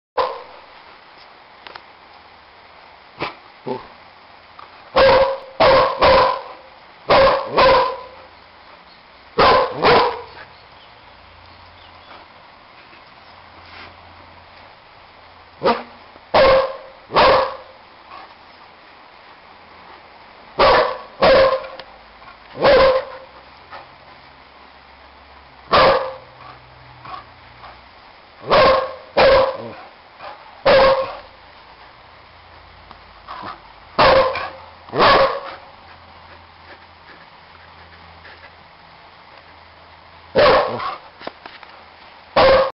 Dog barking at a bird
My doggie Joe barking at a baby bird